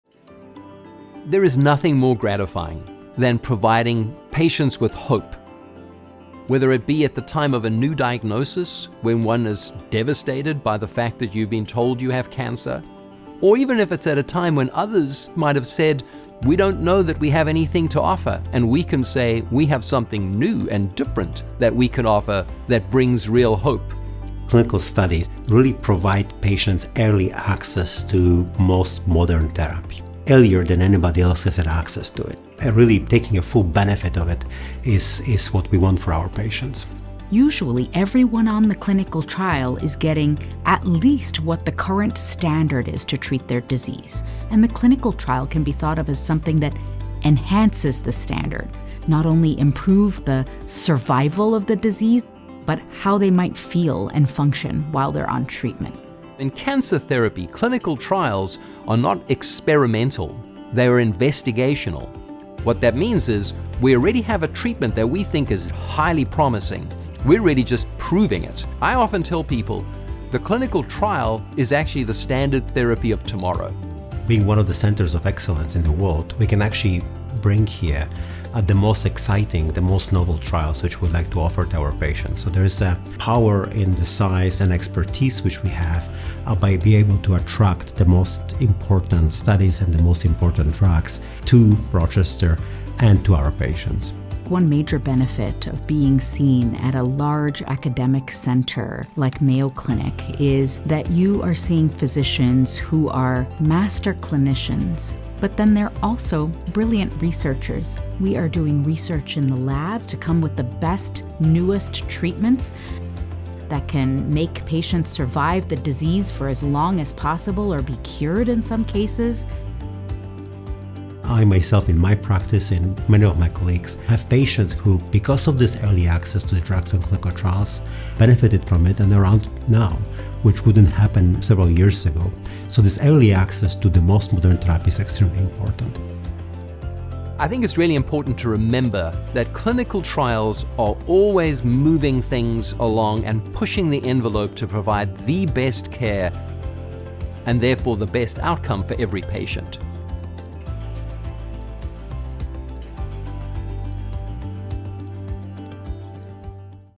Clinical Trials for Blood Cancer
This video discusses how clinical trials provide more hope and options for people diagnosed with a blood cancer such as lymphoma, myeloma or leukemia. Gain a better understanding of the benefits and considerations of enrolling in a clinical study from world-renowned hematologists and researchers who are leading innovative research and helping people with blood cancer live longer, higher quality lives.

For more information about blood cancer care at Mayo Clinic,